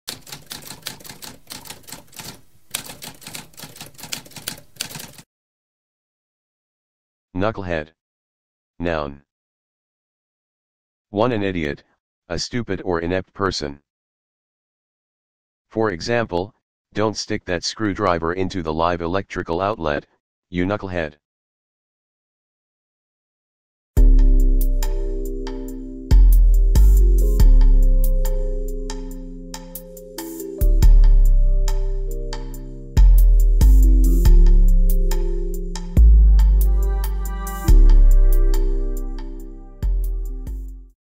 What does knucklehead mean?
A spoken definition of knucklehead.

Intro Sound:
Typewriter - Tamskp